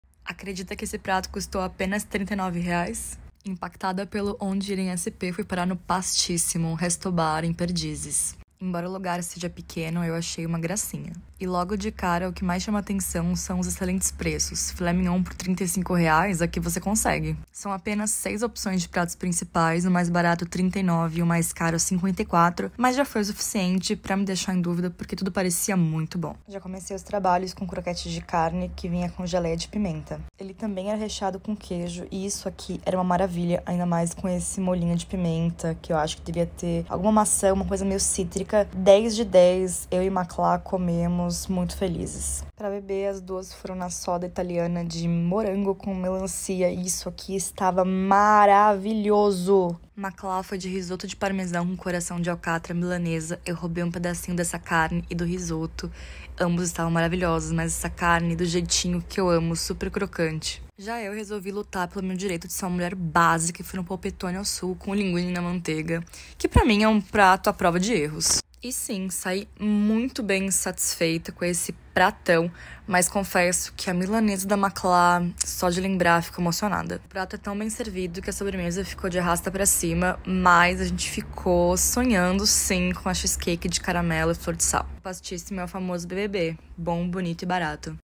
Ste.Sp | Pastissimo Restaurante
Para os amantes de comida italiana boa e no precinho, esse vídeo tem uma dica de milhões (por centavos) ;)